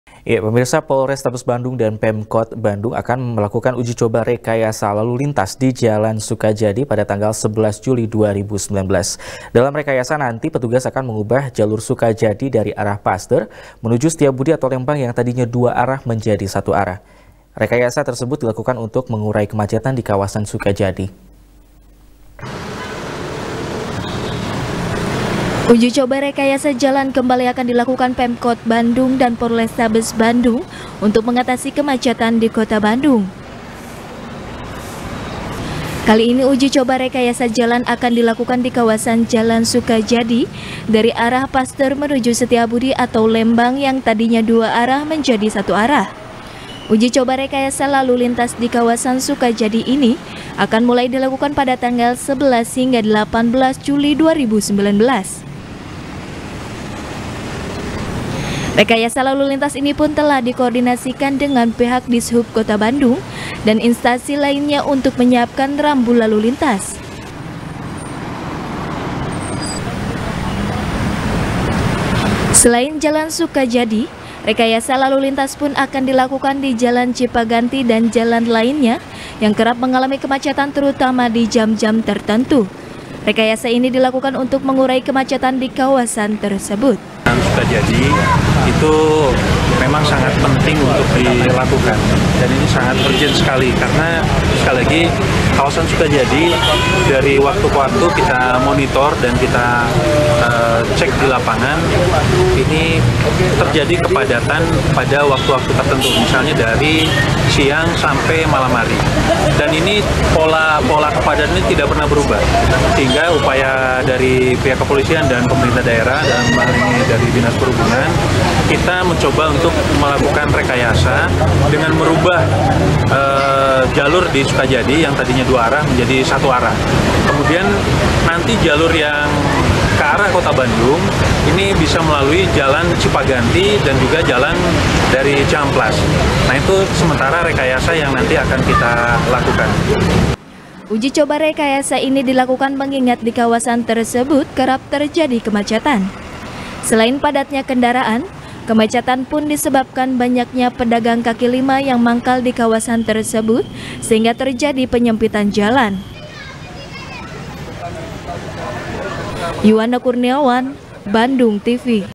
Pemkot Dan Polrestabes Bandung Rekayasa Lalin Jalur Sukajadi | SBR | BANDUNGTV
SBR | BANDUNGTV | IRFAN RAMANDA | Polrestabes Bandung Dan Pemkot Bandung  Akan Melakukan Ujicoba Rekayasa Lalu Lintas, Di Jalan Sukajadi Pada Tanggal 11 Juli 2019. Dalam Rekayasa Nanti, Petugas Akan Mengubah Jalur Sukajadi Dari Arah Pasteur, Menuju Setiabudi Atau Lembang Yang Tadinya Dua Arah Menjadi Satu Arah. Rekayasa Tersebut, Dilakukan Untuk Mengurai Kemacetan Di Kawasan Sukajadi.